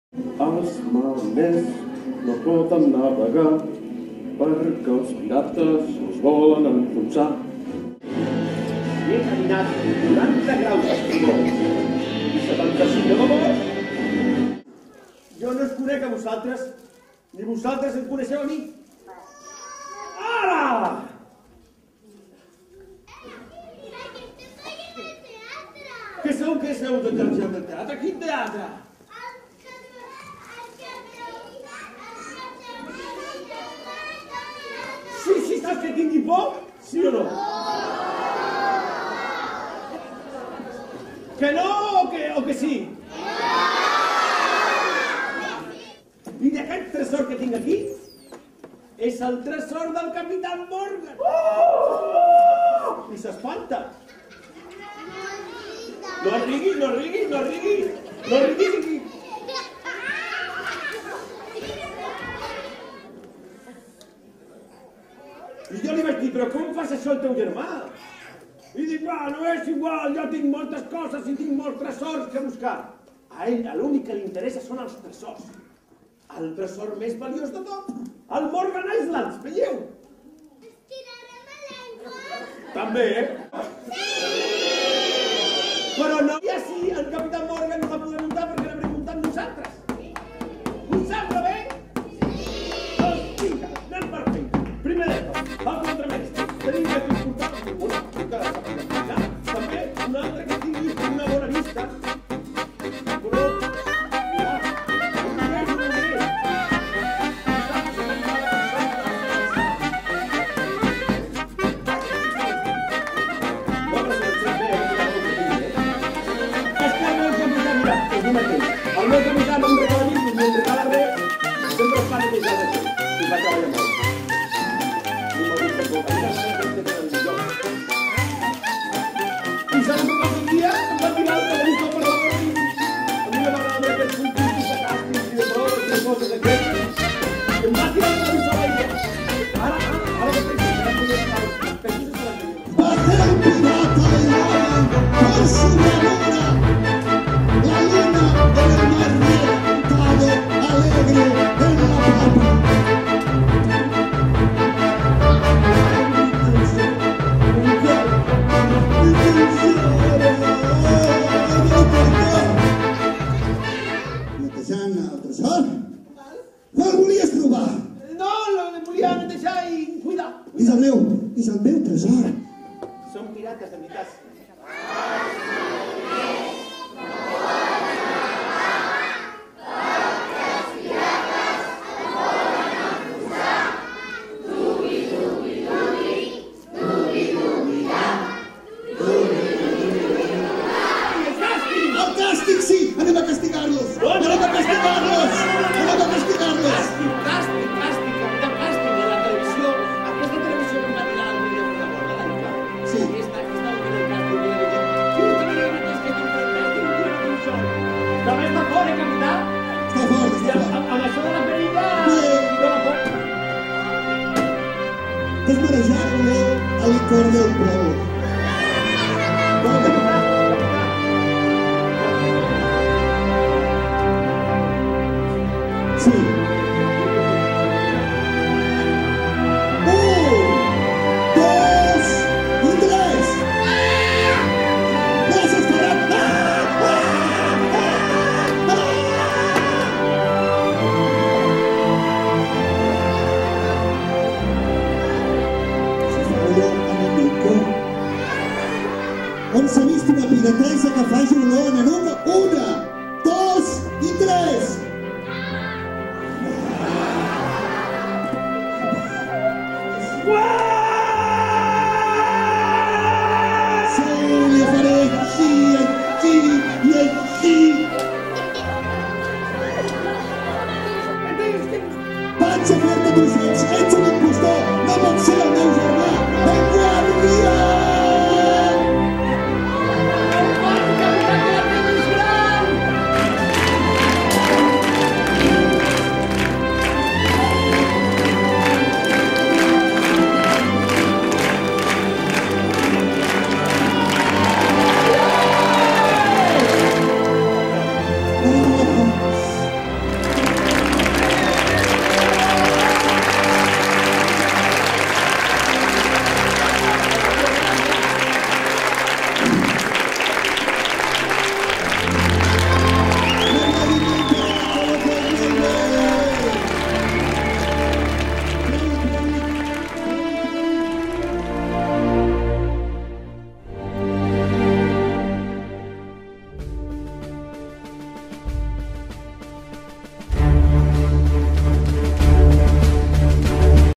Escola de Pirates Teatre Molins de Rei
Mentre el Capità Morgan dormia, el Grumet Timoteu li ha robat el plànol del tresor. Un cop tingui el tresor podrá començar a organitzar la seva pròpia Escola de Pirates. El públic del teatre, pares, mares, nens i nenes tindran l'oportunitat de mostrar la seva vàlua com a pirates. Però el Capità Morgan no ho posarà gaire fàcil i el seu olfacte el durà fins allà on es trobi el seu grumet.
Mientras el Capitán Morgan dormía, el grumete Timoteo le ha robado el plano del tesoro. Timoteo quiere encontrarlo para poder empezar a organizar su propia Escuela de Piratas. El público del teatro, padres, madres, niños y niñas tendrán la oportunidad de mostrar su valía como piratas. Pero el Capitán Morgan no se lo pondrá tan fácil y con su olfato le seguirá á hasta allí donde se encuentre.